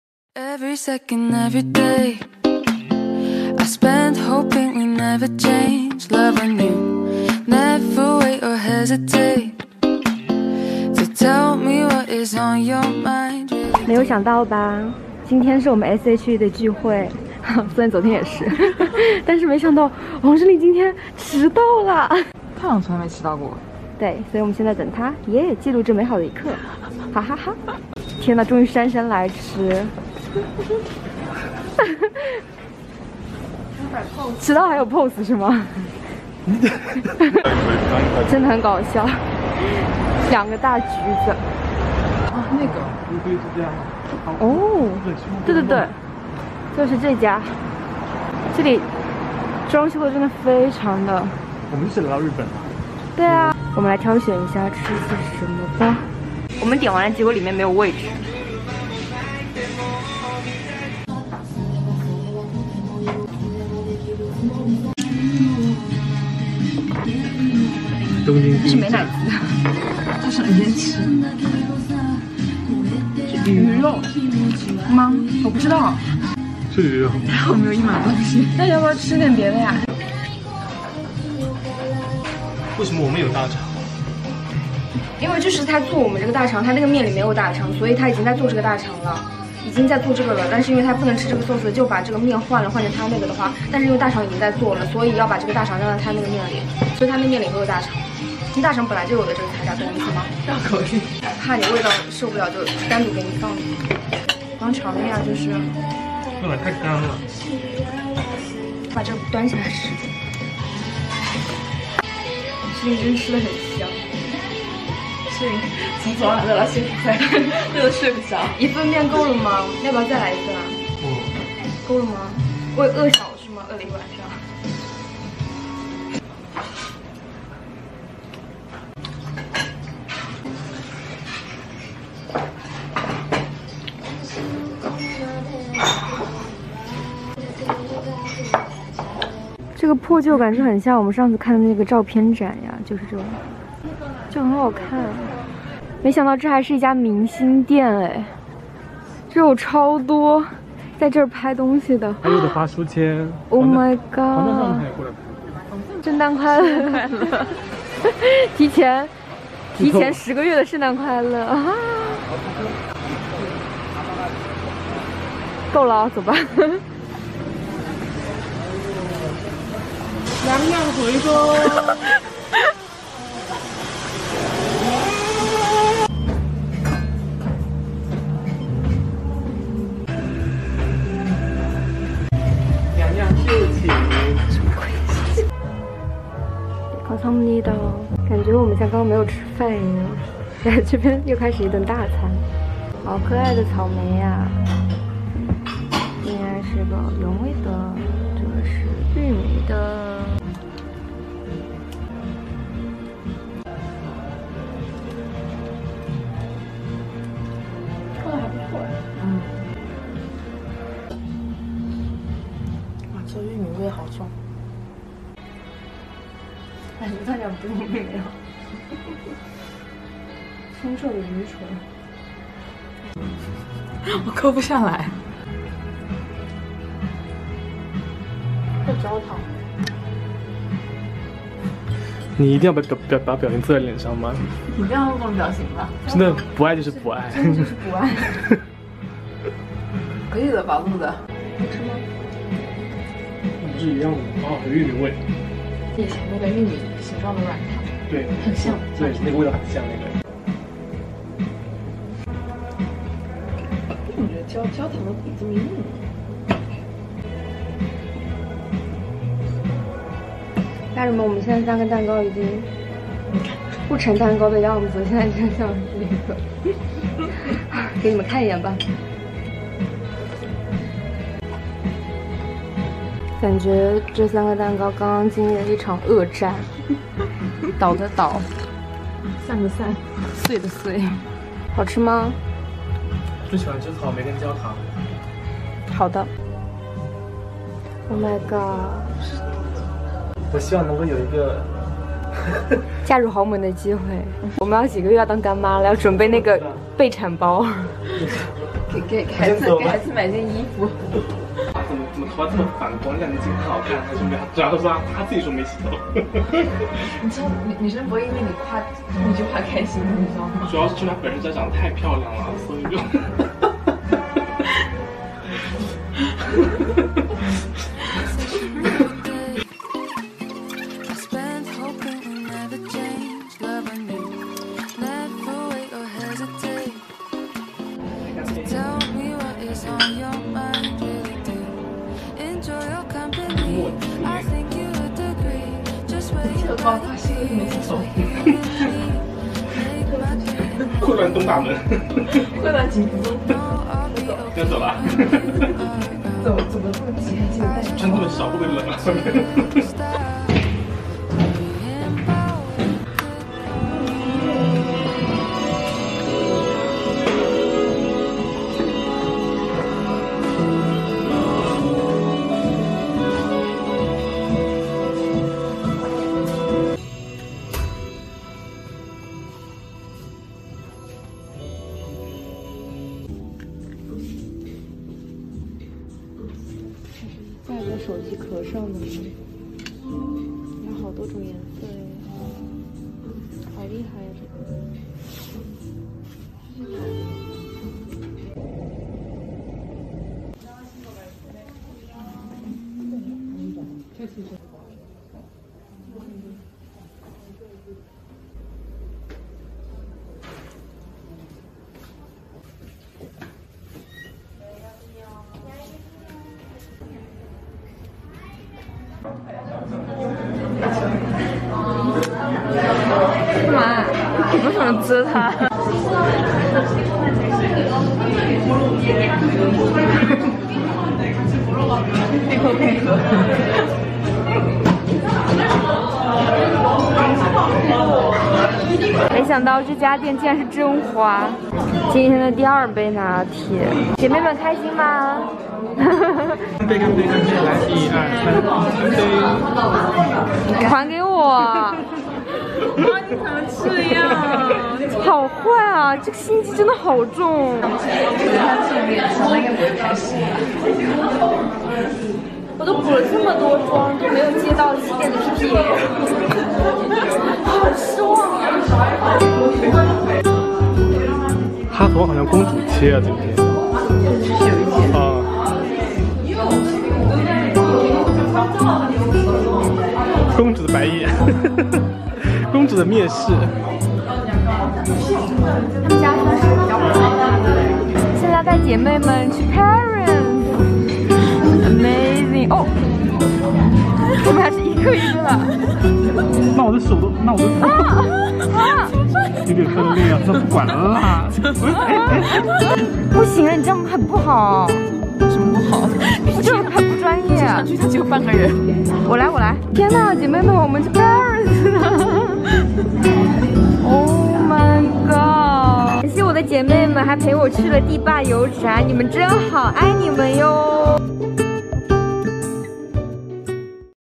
VLOG #160｜韩国留学生｜景福宫探店篇｜好吃的日式拉面｜出门必去的咖啡店｜和姐妹在一起的时光｜说废话也很开心
生活这件小事,咖啡,探店,韩国,生活记录,独居,网红,记录,好吃,景福宫,生活万花筒5.0·生活这件小事